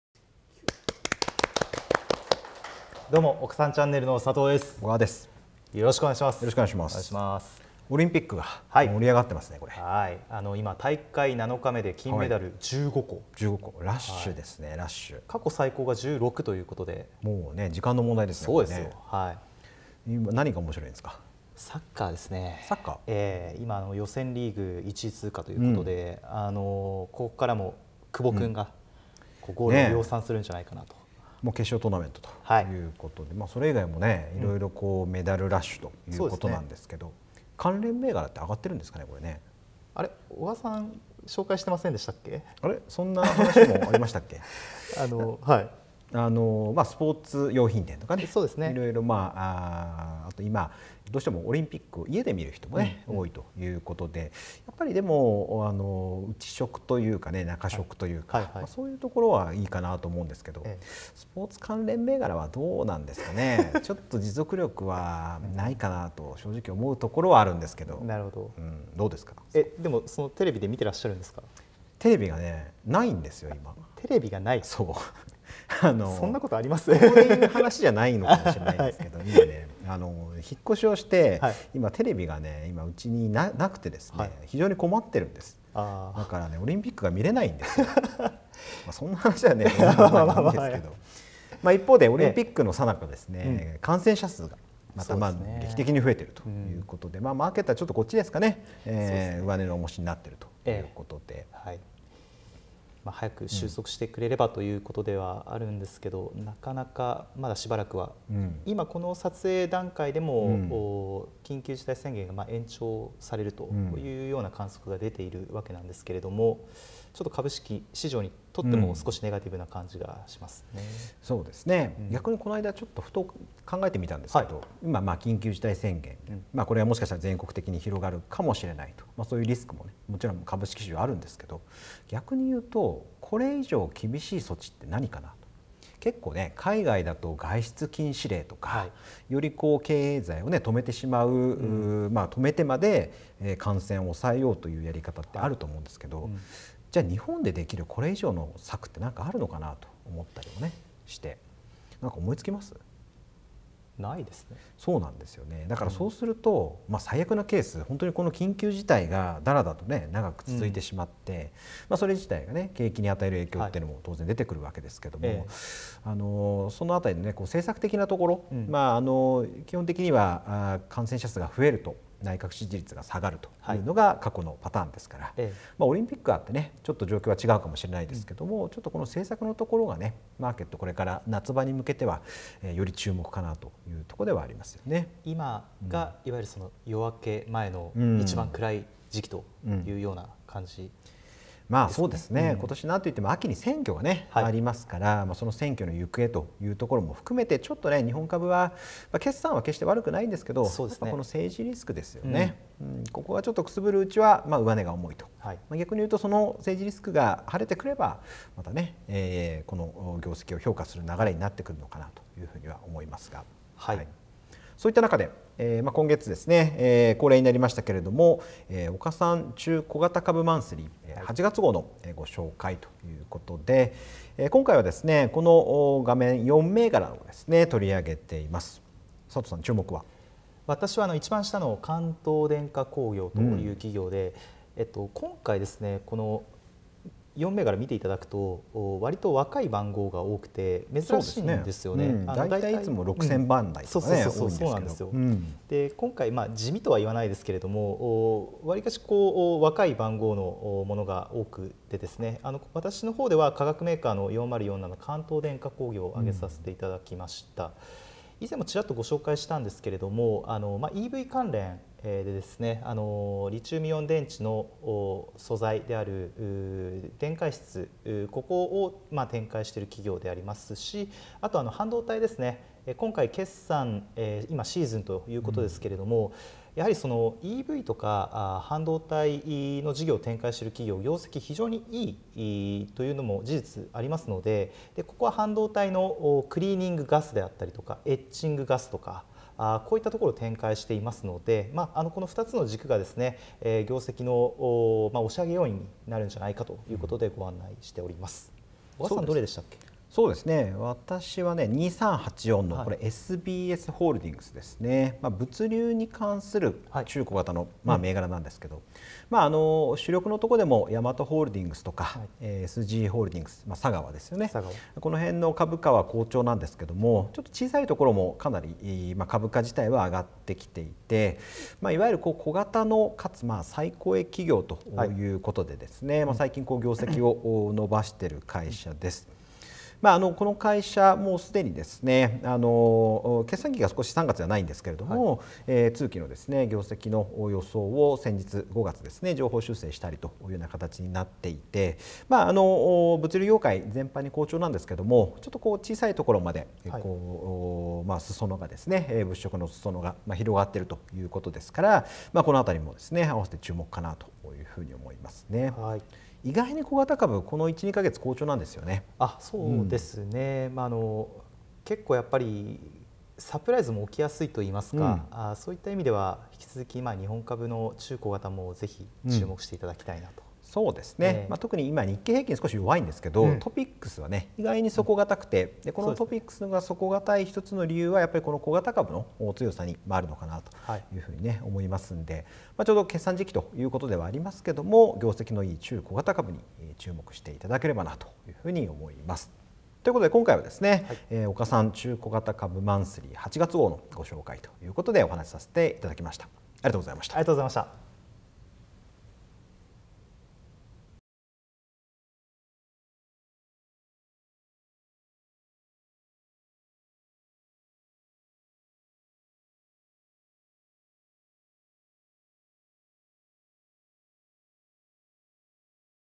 岡三ちゃんねる#24 中小型株マンスリー2021年8月号【岡三証券】小川佳紀と佐藤健による相場解説！ 関東電化工業 SBS
～ほぼ毎週月曜更新～
次回更新は8月9日（月）予定となります、お楽しみに！

岡三証券の投資戦略部 日本株戦略グループ 小川と佐藤による
注目の銘柄や相場のポイントを解説する番組
【岡三ちゃんねる】略して【おかちゃん】第24回をお届けします！
今回のテーマは　「中小型株マンスリー2021年8月号」です。

今回の注目銘柄は
佐藤　4047　関東電化工業
小川　2384　SBSHD　です。

※動画の最後に重要なお知らせがございます。
　どうぞ最後までご確認ください。

前回の第23回テーマ「再生可能エネルギー！」はこちら！

資産運用のご相談なら岡三証券

【その他Web動画セミナーも、岡三証券ホームページ内にて随時更新中】